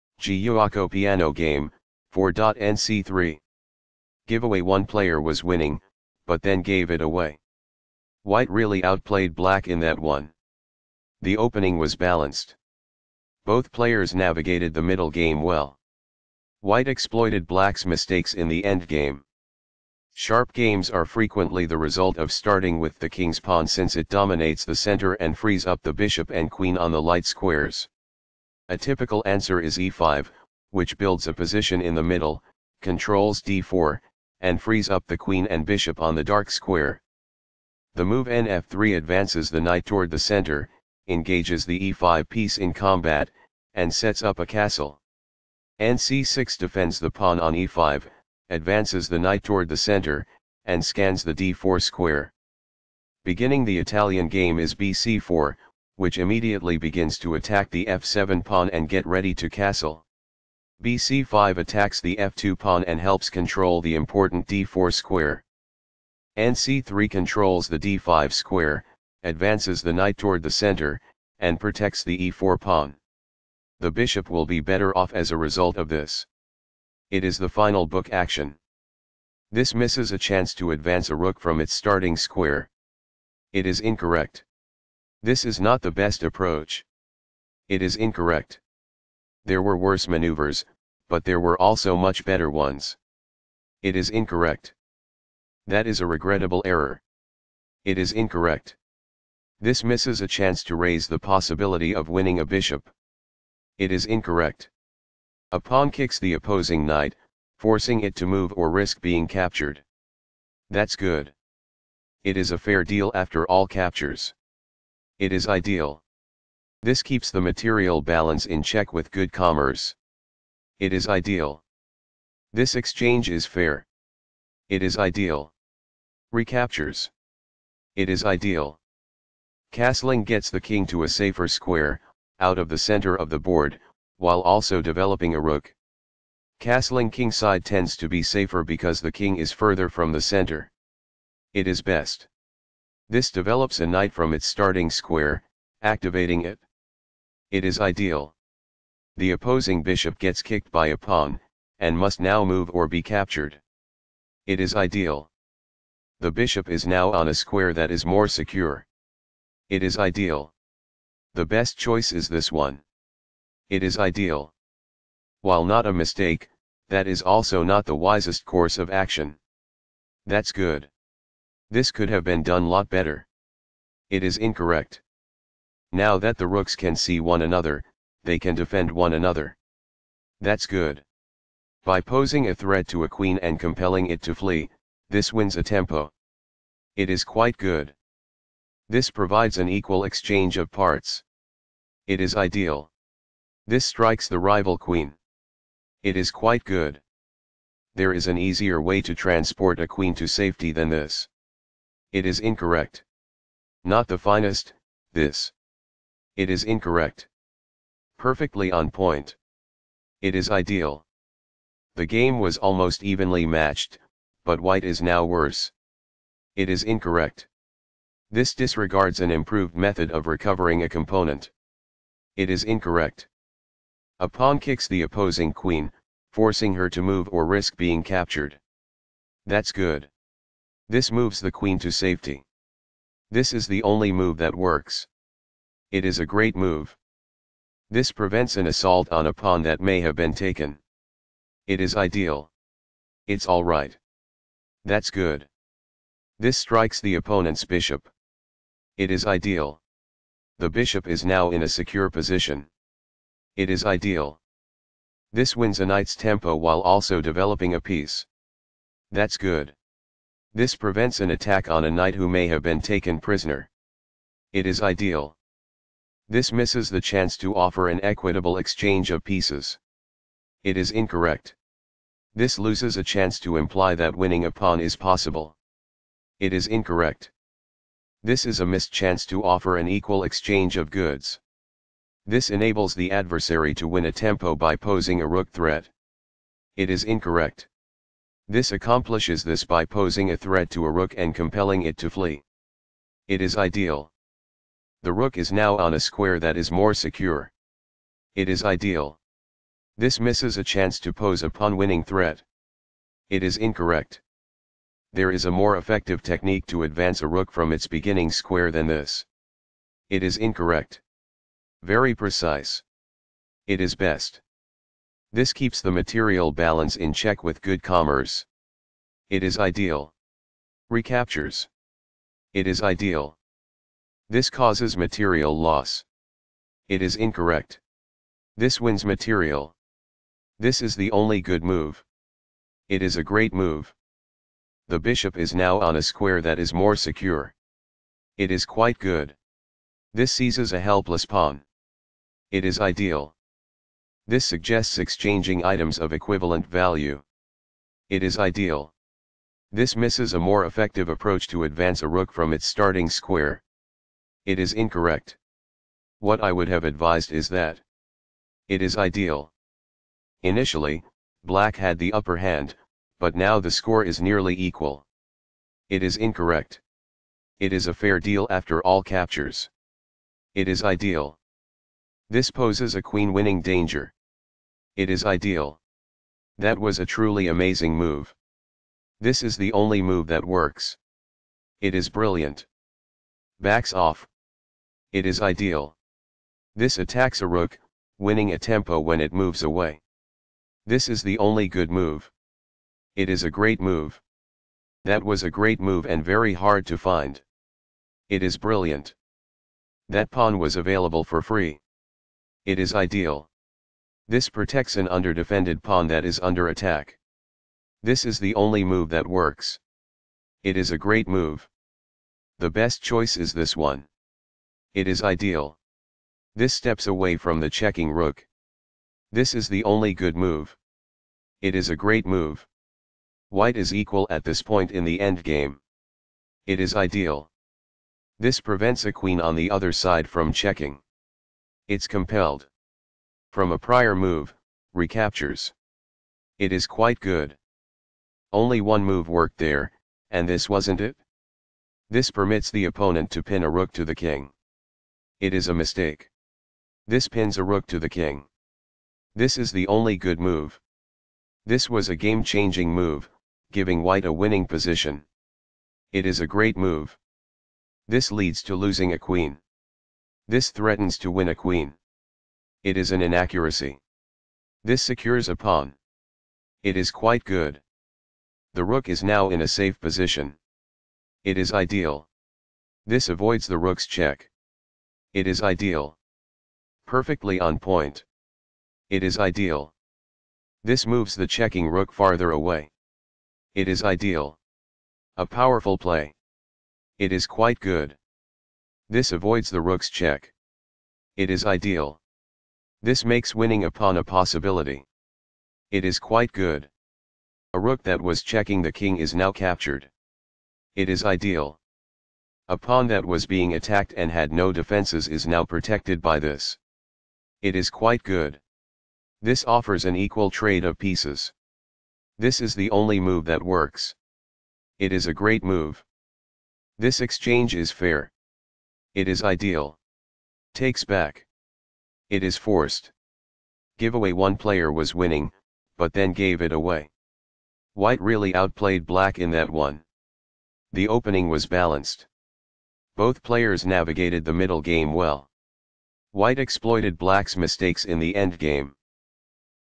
White Morozevich Alexander (RUS), Black Ivanchuk Vassily (UKR),Giuoco Piano Game: 4.Nc3, Event It (
Giuoco Piano Game: 4.Nc3, Event It (active), Site Yalta (Ukraine), Date 1995, Round 3, White Morozevich Alexander (RUS), Black Ivanchuk Vassily (UKR), Result 1-0, ECO C50, WhiteElo 2630, BlackElo 2740, Remark VI